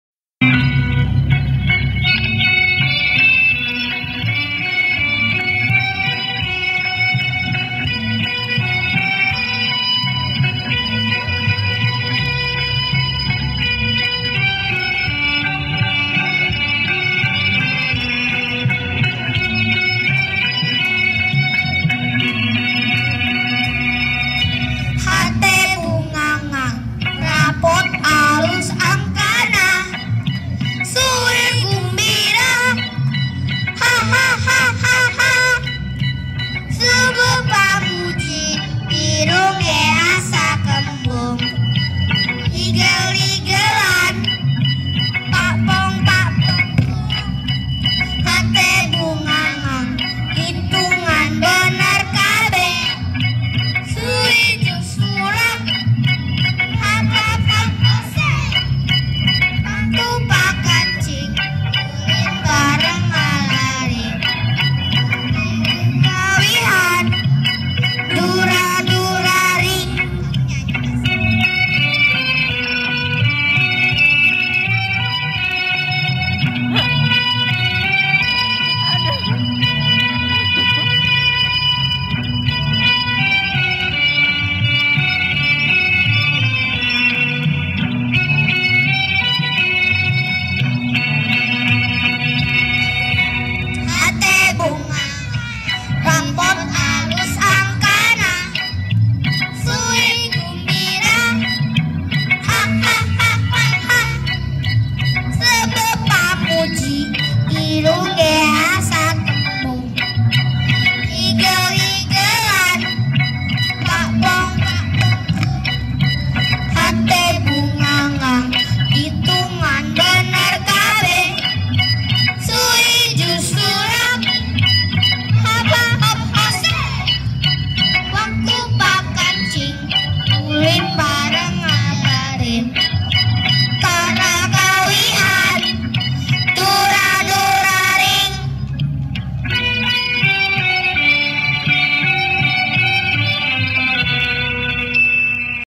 Gumbira
Pentas Seni yang dibawakan oleh SD kelas 2  Permata Bunda